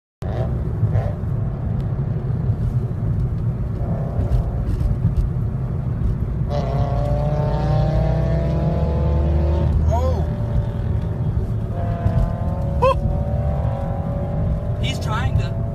Freight Truck Merges into BRZ? **Close Call**
Freight truck seen moving into occupied lane? Well let you be the judge! Let us know whom you think would be at fault. Thankfully all crisis averted.

CREDIT: @trini_stock_wrx9686